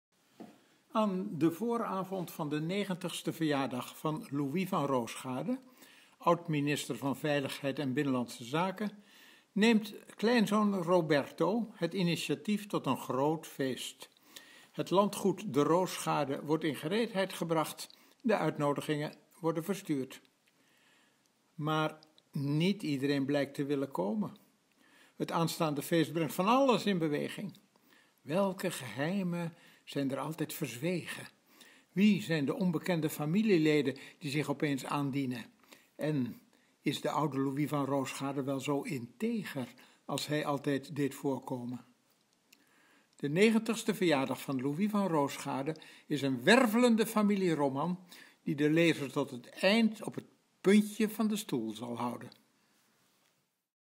Jan Terlouw leest flaptekst DE 90STE VERJAARDAG VAN LOUIS VAN ROOSGAARDE
WERVELENDE FAMILIEROMAN VOL GEHEIMEN EN ONTHULLINGEN
Aan de vooravond van de negentigste verjaardag van Louis van Roosgaarde, oud-minister van Veiligheid en Binnenlandse Zaken, neemt kleinzoon Roberto het initiatief tot een groot feest. Het landgoed De Roosgaarde wordt in gereedheid gebracht, de uitnodigingen verstuurd.
Maar niet iedereen blijkt te willen komen. Het aanstaande feest brengt van alles in beweging. Welke geheimen zijn er altijd verzwegen, wie zijn de onbekende familieleden die zich opeens aandienen, en is de oude Louis van Roosgaarde wel zo integer als hij altijd deed voorkomen?
DE 90STE VERJAARDAG VAN LOUIS VAN ROOSGAARDE is een wervelende familieroman die de lezer tot het einde op het puntje van de stoel zal houden.